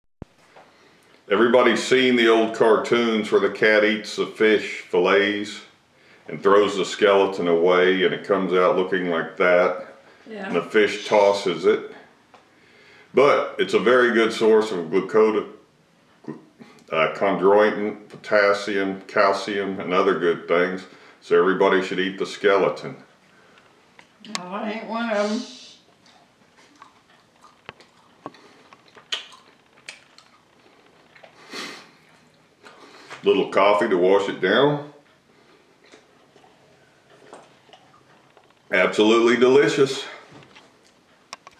Man eats skeleton
Bones and joints are a very good source of Glucosamine & Chondroitin, calcium, and connective tissue.